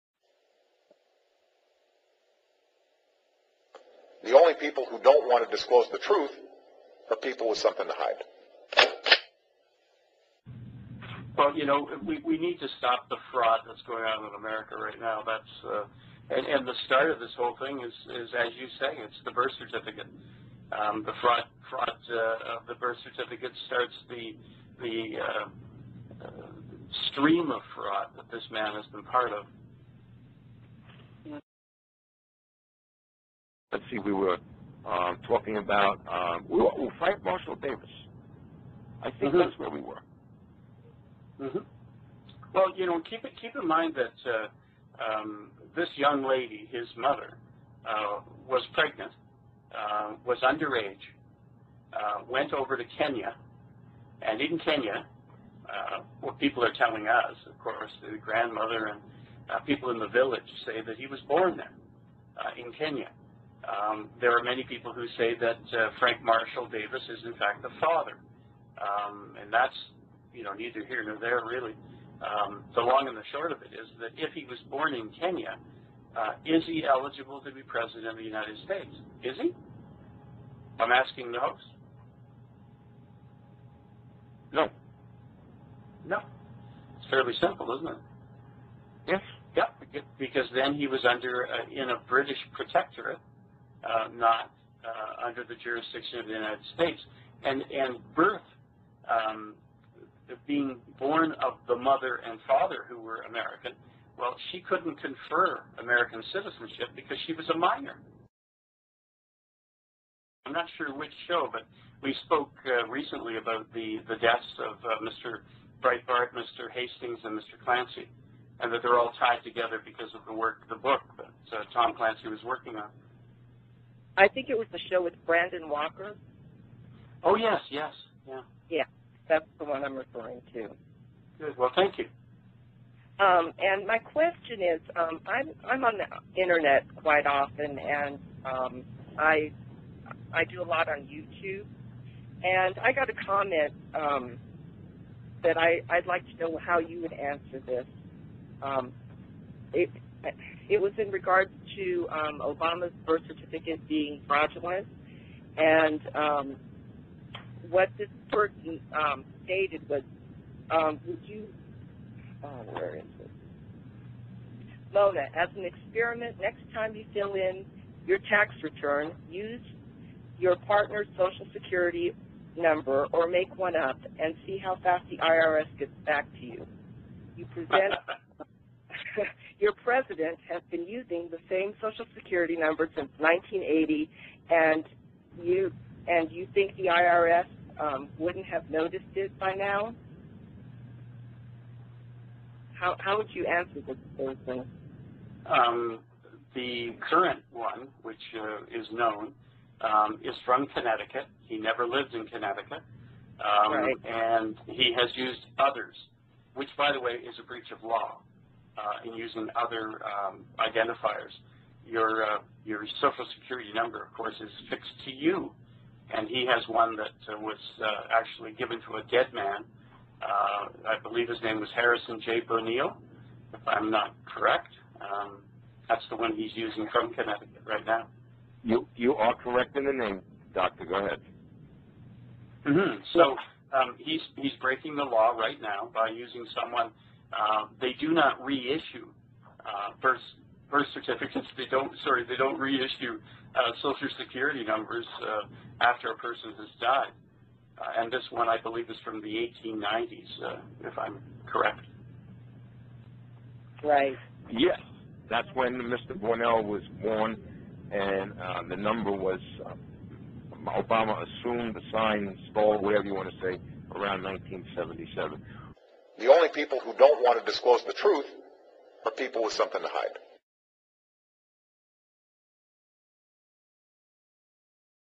Ex CIA Operative Dr Garrow Obama has dead mans social security number from 1890's
Credit:Fox News